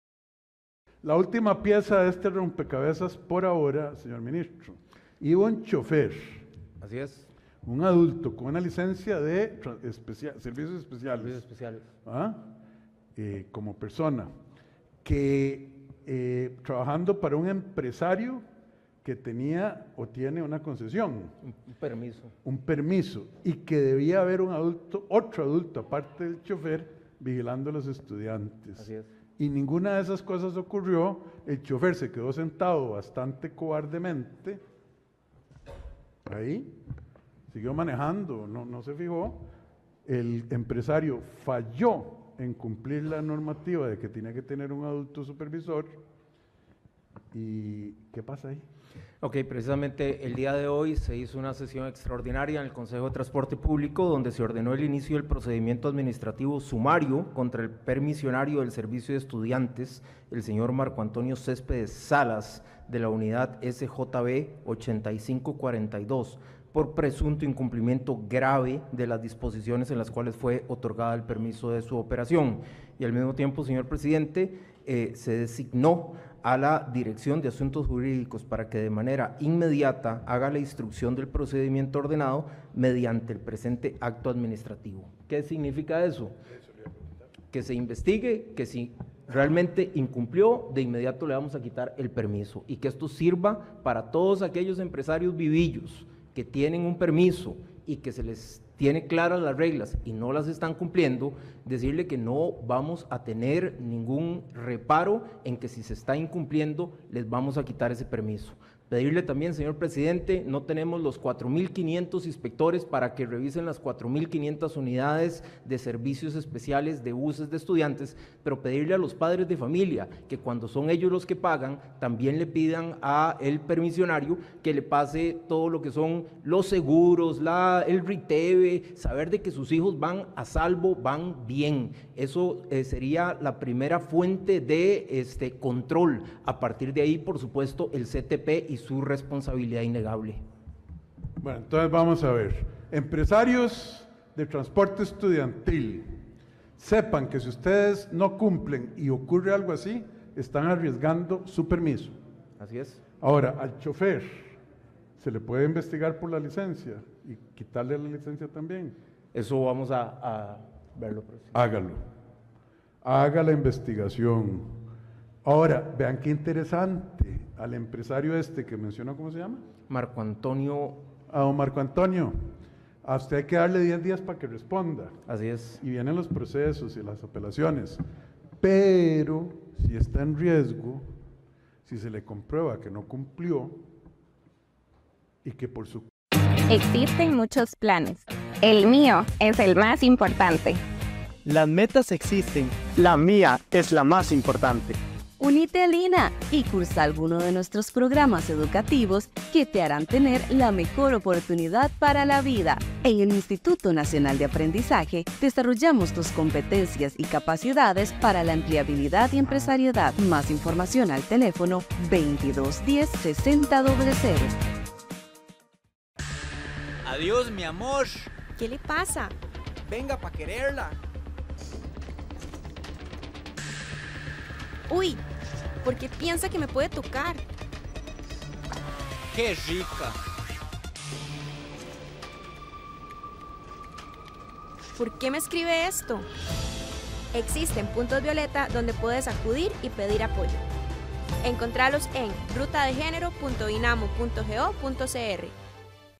MOPT le puso la Mira a los transportes de estudiantes, caso Valentina
Actualidad de Costa Rica y el mundo.